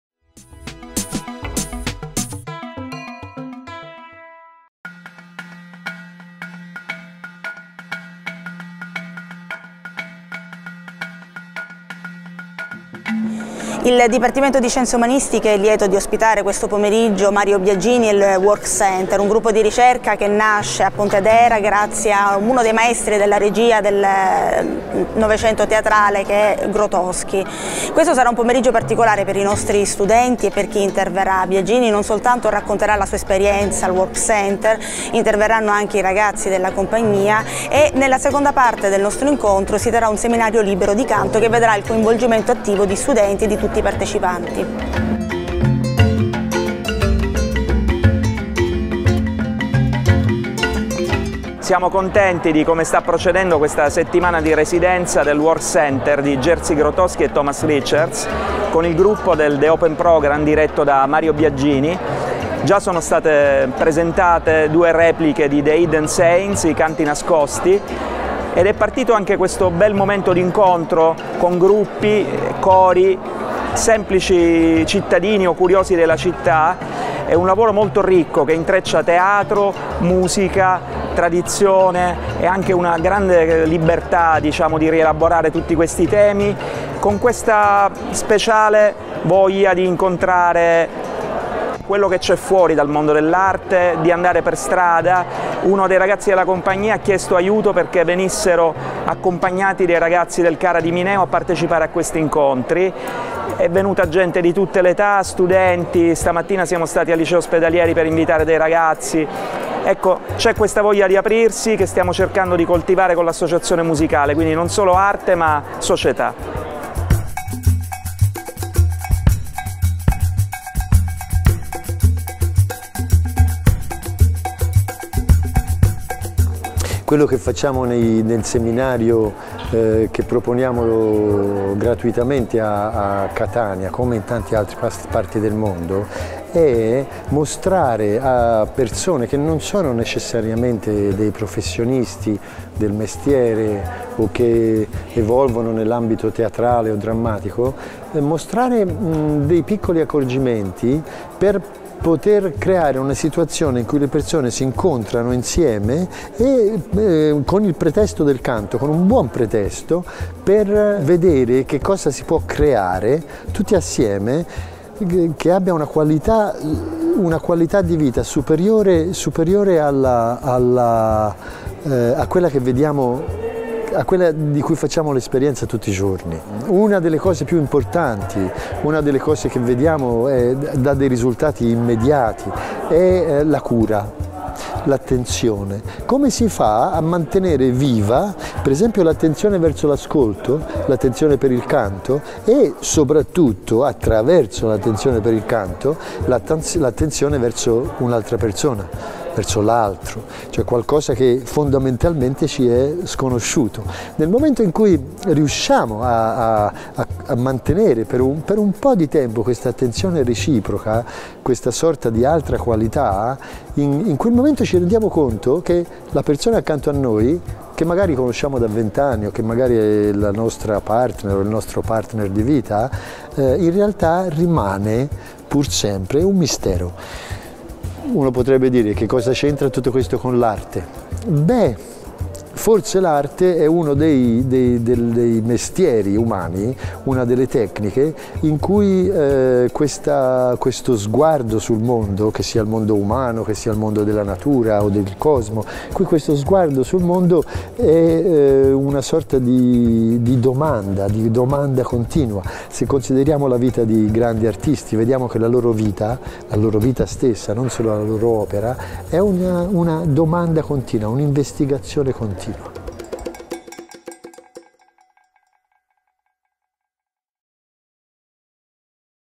Uno sguardo sul Workcenter
Venerdì 25 gennaio 2019 l'auditorium "De Carlo" del Monastero dei Benedettini (Dipartimento di Scienze umanistiche dell'Università di Catania) ha ospitato il Workcenter di Jerzy Grotowski e Thomas Richards, uno dei centri di ricerca teatrale più importanti e riconosciuti a livello mondiale. Dal 1999 il direttore artistico Richards e il direttore associato Mario Biagini - unici eredi del patrimonio di Grotowski che include l'intero corpus dei suoi scritti - proseguono il percorso di ricerca sulla performance da lui avviato.
Nel corso del seminario, promosso dal Disum in collaborazione con l'Associazione musicale etnea, sono stati proiettati frammenti filmati di opere e lavori del Workcenter dal 1989 al 2019, accompagnati dagli interventi di Mario Biagini e degli attori, della docente Simona Scattina (Disum), di Biagio Guerrera e Luca Recupero (Ame) e di Matilde Politi e Simona Di Gregorio (Coro Unicavuci). A seguire si è poi tenuto un seminario libero "Invito al canto".